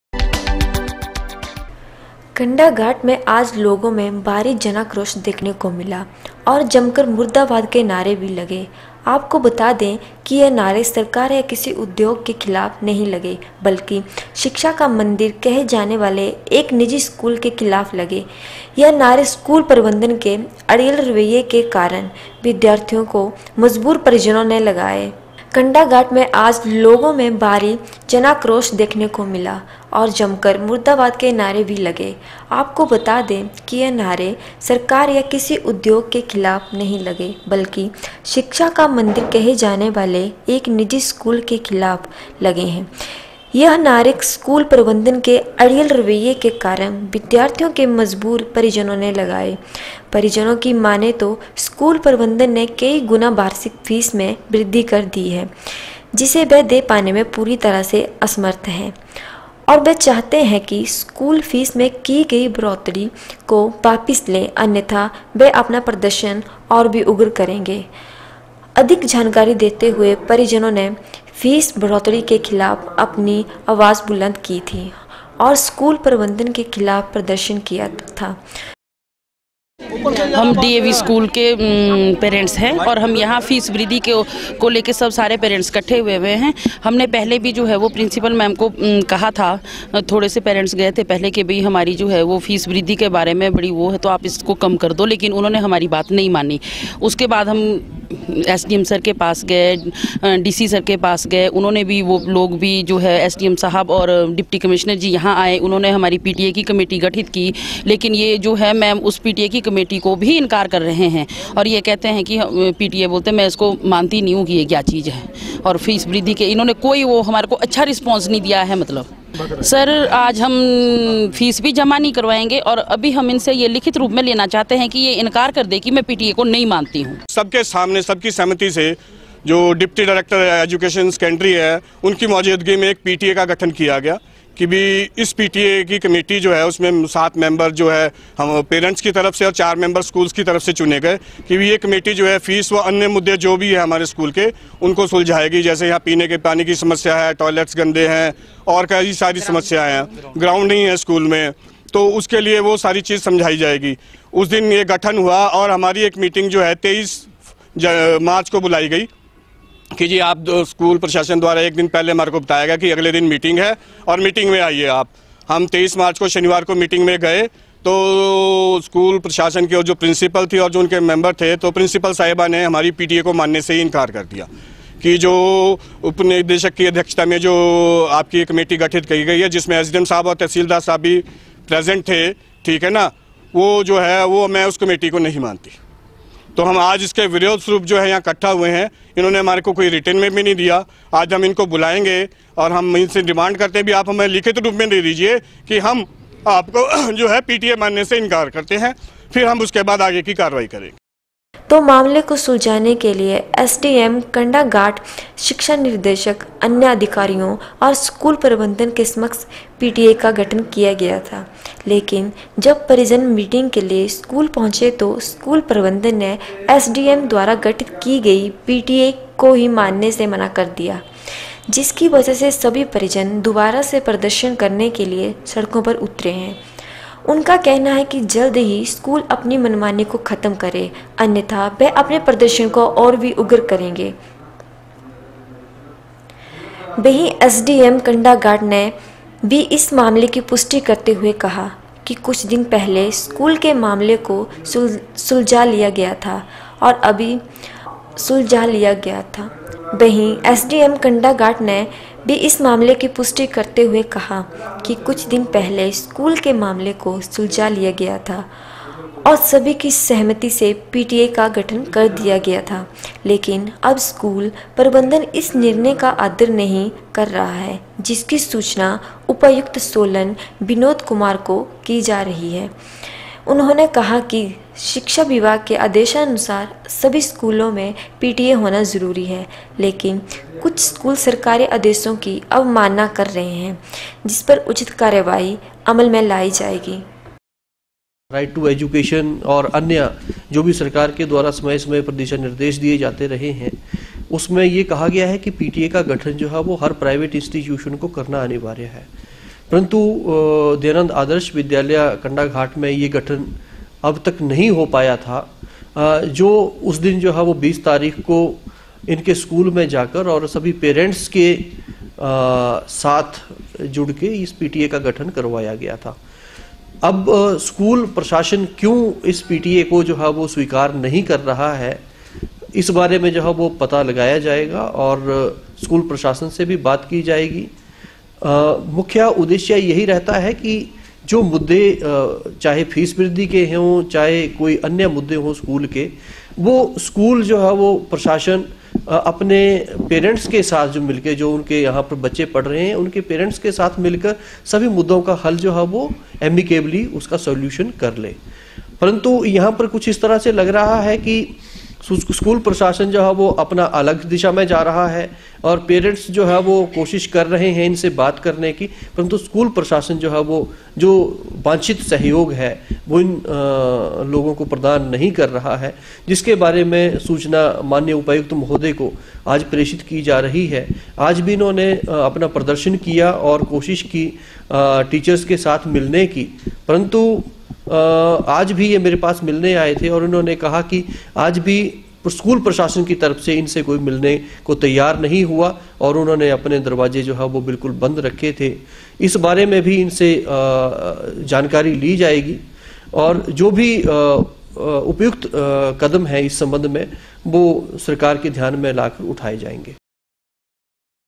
प्रशासनिक अधिकारियों के आदेशों को भी नहीं मान रहा स्कूल
कंडाघाट में आज लोगों में भारी जनाक्रोश देखने को मिला और जम कर  मुर्दा बाद के नारे  भी लगे  | आप को बता दें कि यह नारे सरकार या किसी उद्योग के खिलाफ नहीं लगे बल्कि शिक्षा का मन्दिर कहे जाने वाले एक निजी स्कूल के खिलाफ लगे है  यह नारे स्कूल प्रबन्धन के अड़ियल रवैये के कारण  विद्यार्थियों के  मजबूर परिजनों ने लगाए | परिजनों की माने तो स्कूल प्रबन्धन ने कई गुना वार्षिक फीस में वृद्धि कर दी है जिसे वह दे पाने में पूरी तरह से असमर्थ है और वह चाहते है कि स्कूल  फीस में की गई बढौतरी को वापिस लें अन्यथा वह अपना प्रदर्शन और भी उग्र करेंगें |